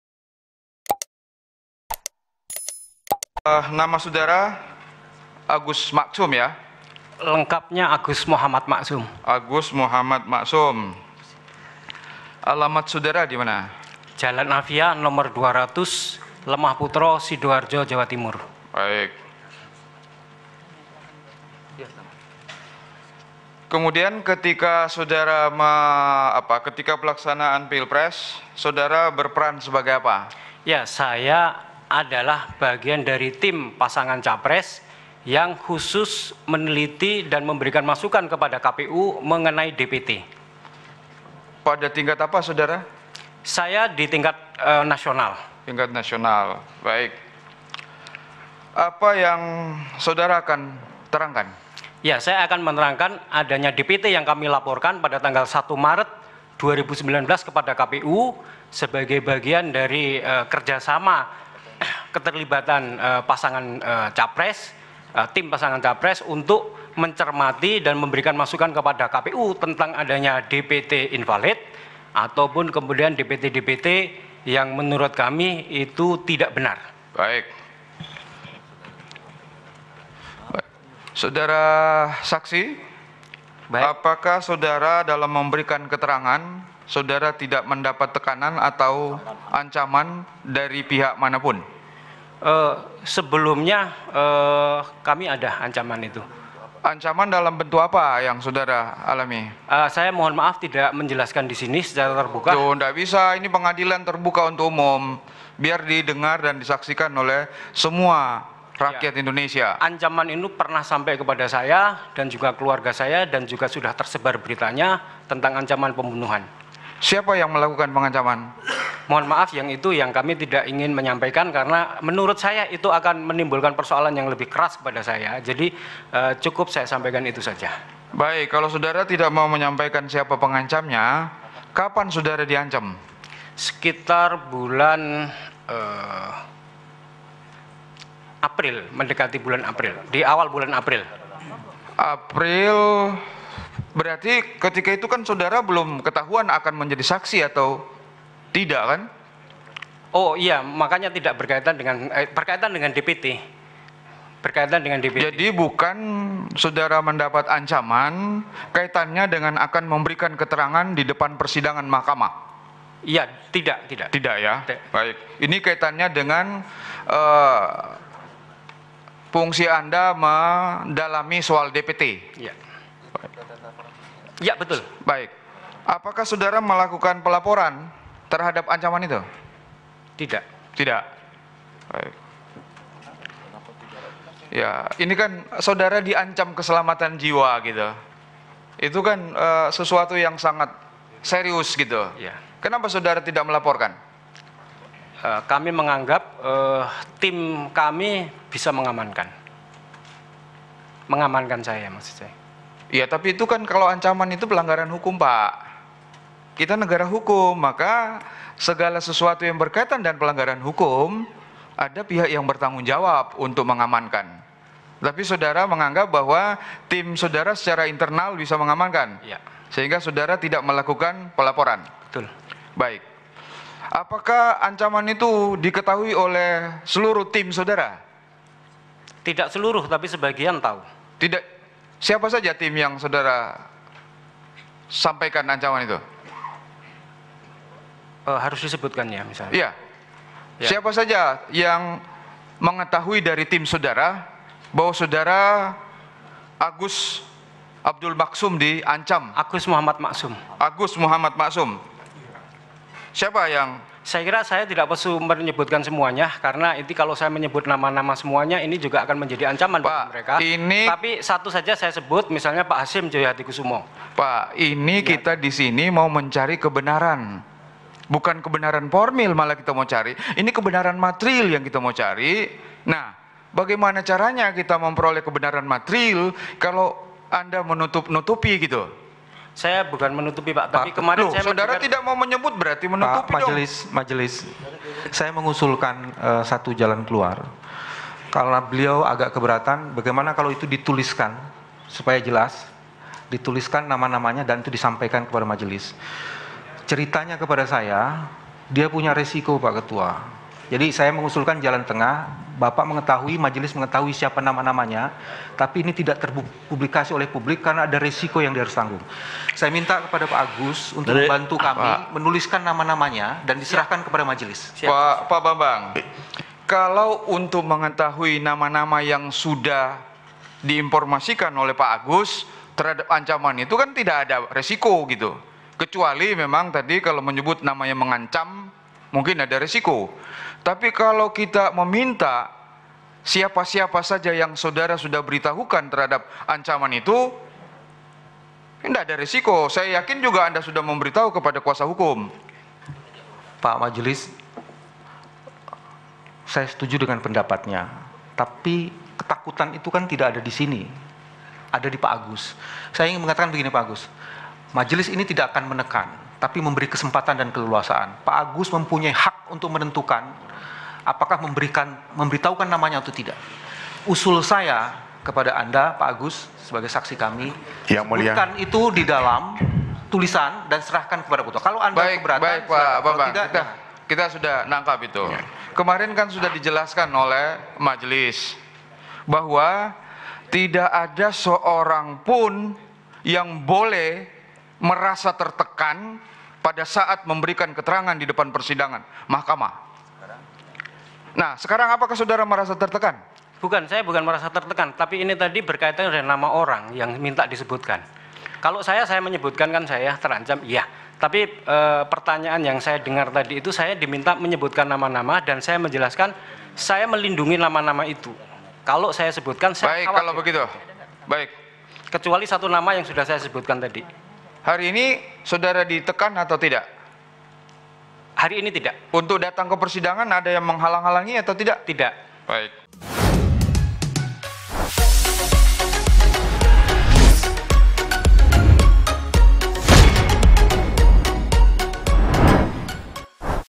Saat Bersaksi di MK, Saksi Tim Prabowo Mengaku Diancam Dibunuh
Sidang ketiga sengketa pilpres 2019 berlangsung di kawasan gedung Mahkamah Konstitusi, Jalan Merdeka Barat, Jakarta, Rabu (19/6). Pada agenda hari ini, MK mendengarkan keterangan saksi dan ahli yang diajukan oleh pemohon, Prabowo Subianto-Sandiaga Uno.

Dalam sidang, tim hukum Prabowo Subianto menghadirkan Agus Maksum sebagai saksi. Agus merupakan timses Prabowo yang bertugas memberikan masukan ke KPU tingkat nasional. Hakim konstitusi Aswanto bertanya kepada saksi Agus Maksum mengenai ancaman pembunuhan.

Ancaman pembunuhan itu kerap disuarakan oleh tim hukum Prabowo, Bambang Widjojanto (BW). Berikut keterangan lengkapnya.(*)